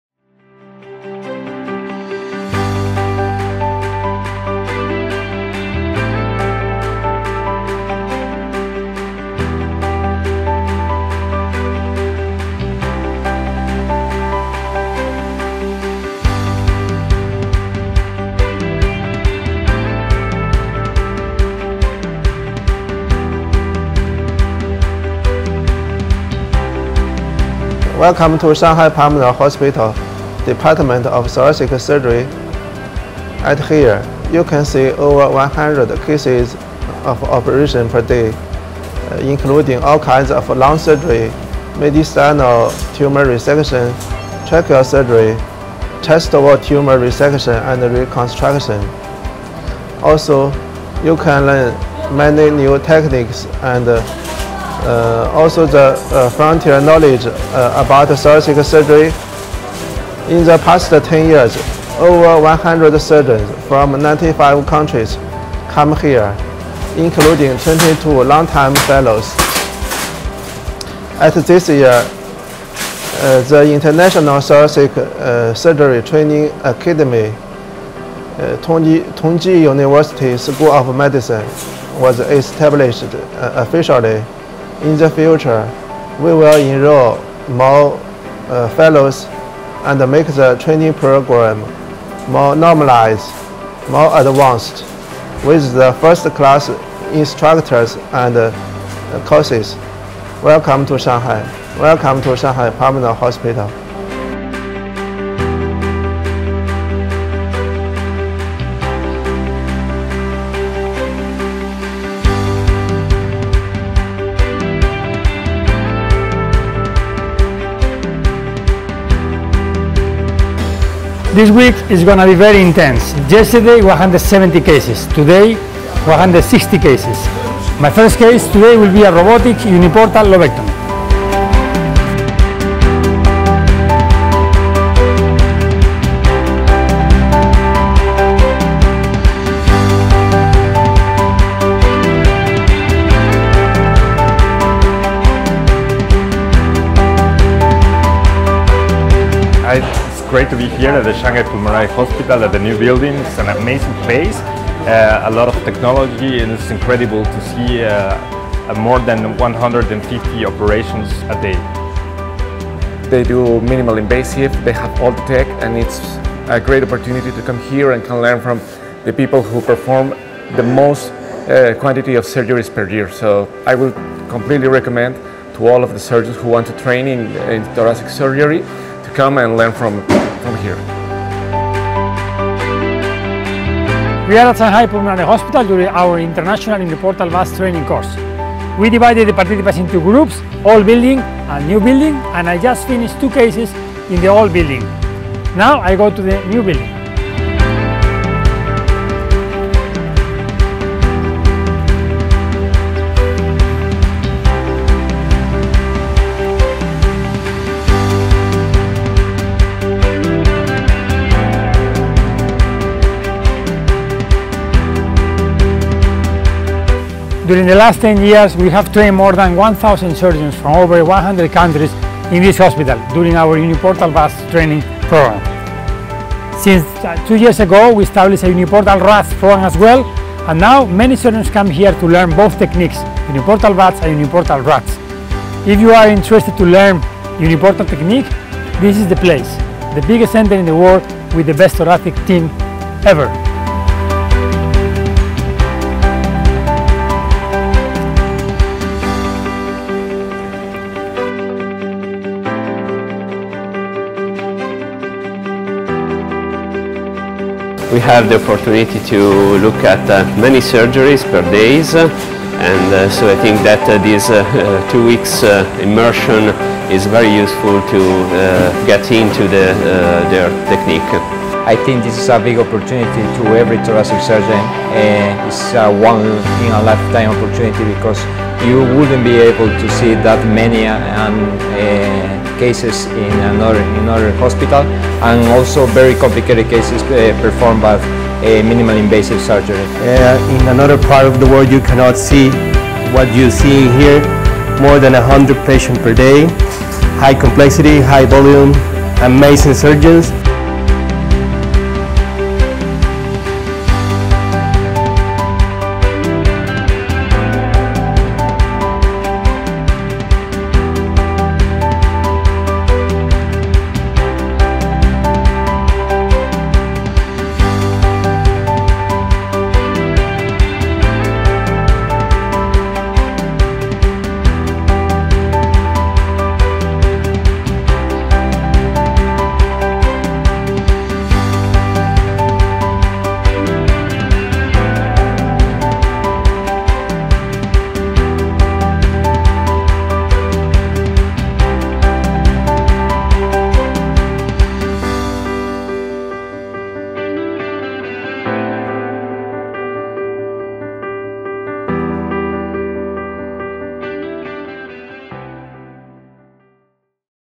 UNIPORTAL VATS AND RATS SHANGHAI TRAINING PROGRAM (MAY 2024)
Video of Uniportal VATS and RATS training programa at Shanghai Pulmonary Hospital during May 2024. The biggest thoracic center in the world. 20 surgeons coming from all around the world spend 2 weeks with us learning everything about the uniportal technique. Lectures and an average of 150 cases per day.
Video by Daniel Lopez